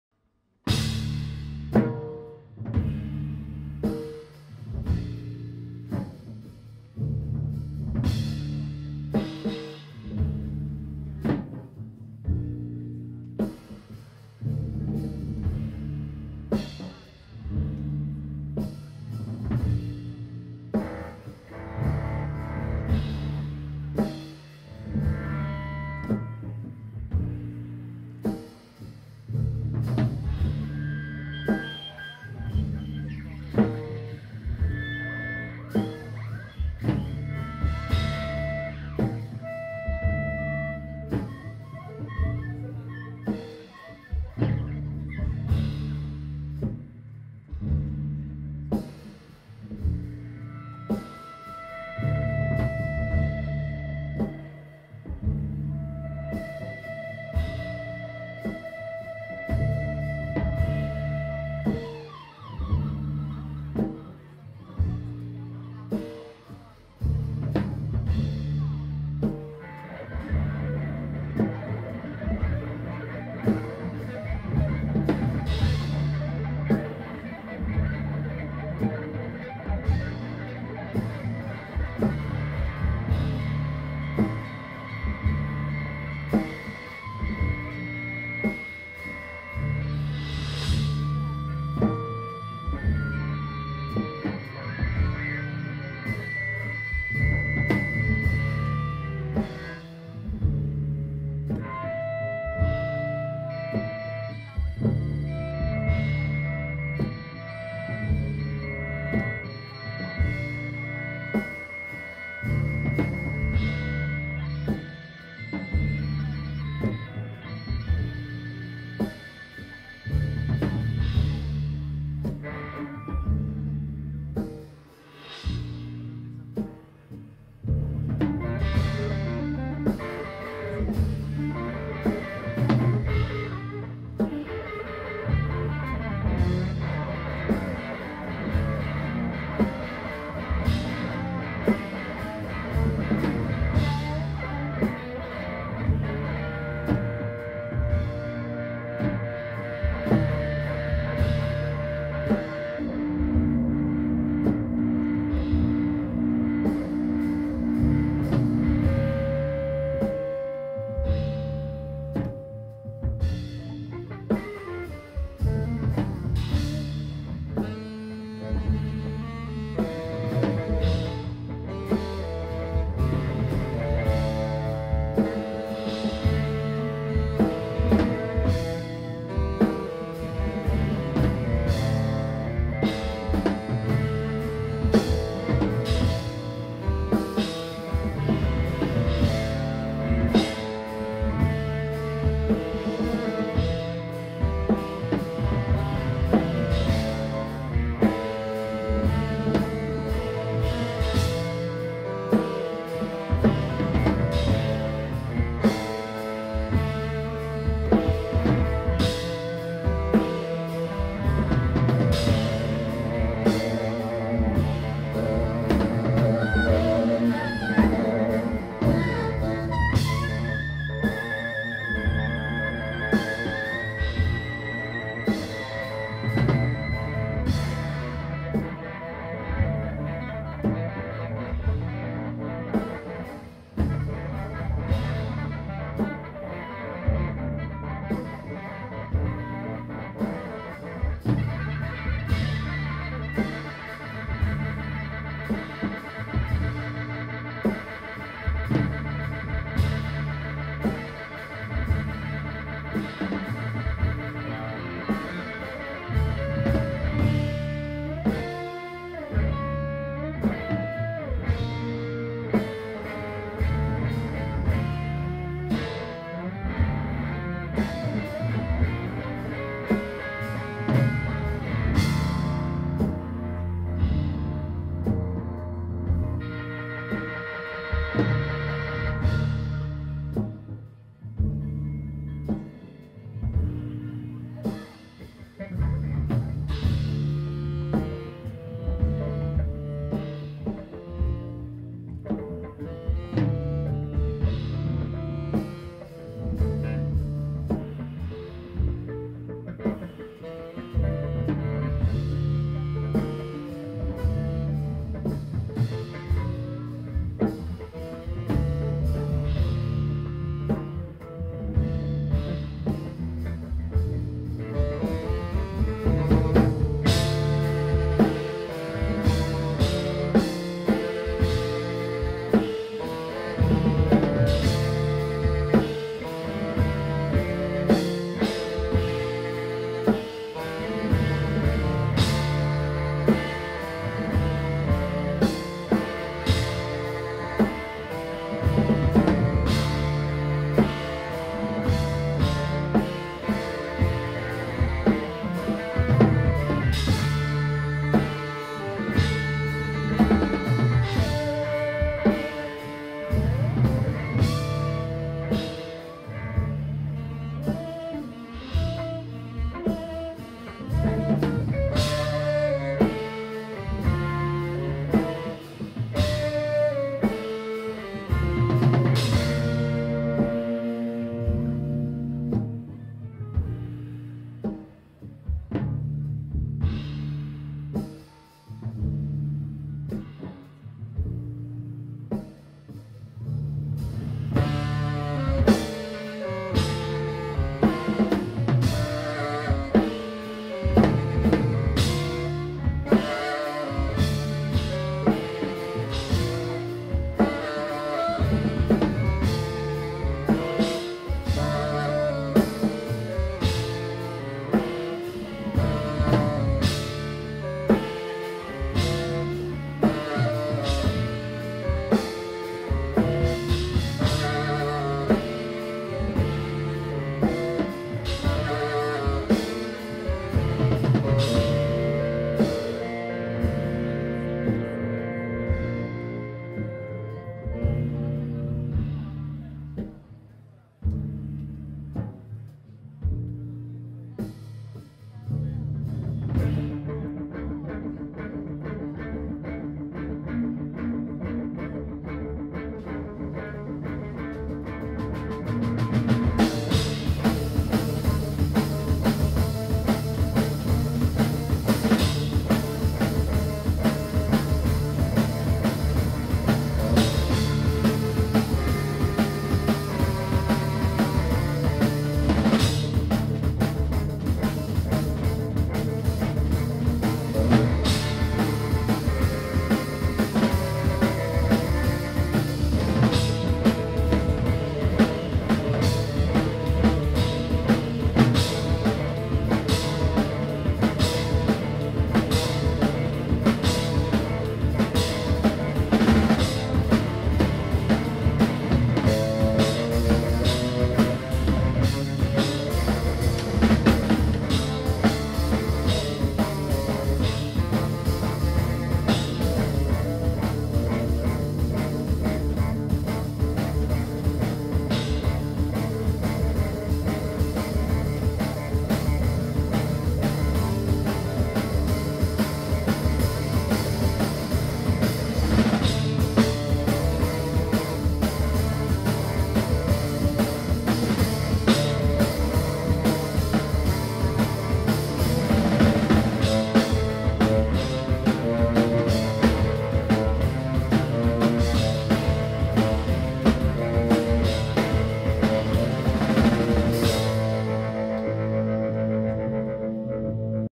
Caterina Palazzi Quartet SUDOKU KILLER live @ Init 3/05/2015
Audio registrato con volume di input basso per evitare distorsioni. Alzate il volume del vostro stereo/pc :-)

Low input volume to avoid clipping. Turn up your stereo/pc volume.